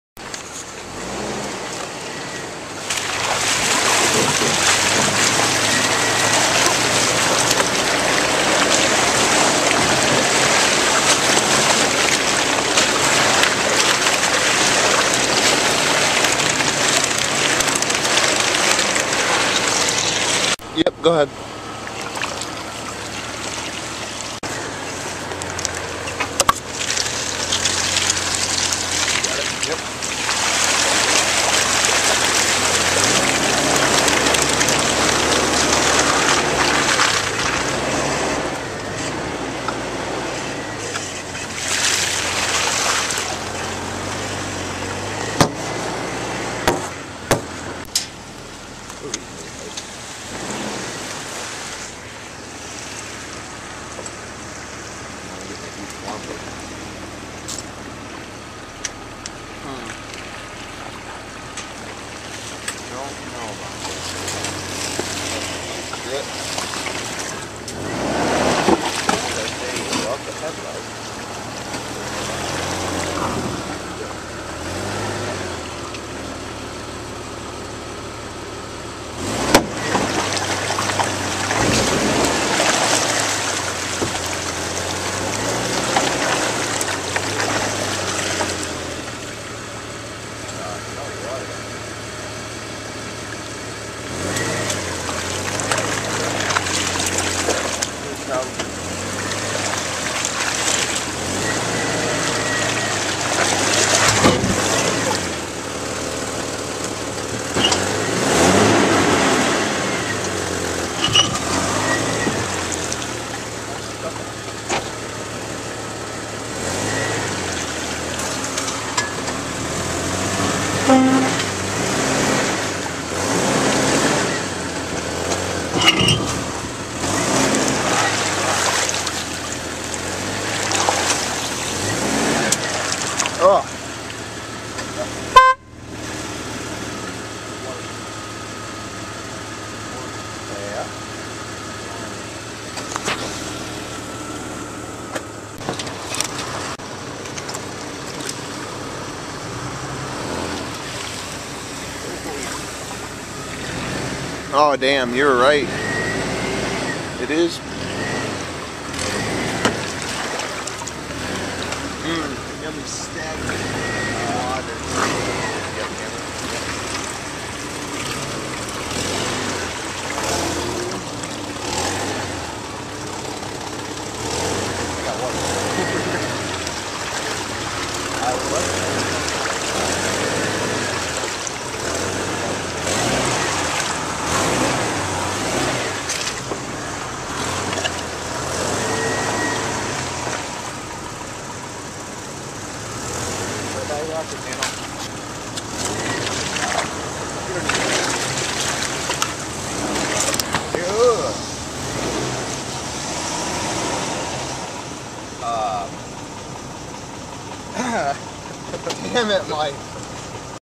Day Out Wheeling...
Had an awesome day out driving through snow and water and getting stuck a few times...enjoy...more videos to be uploaded later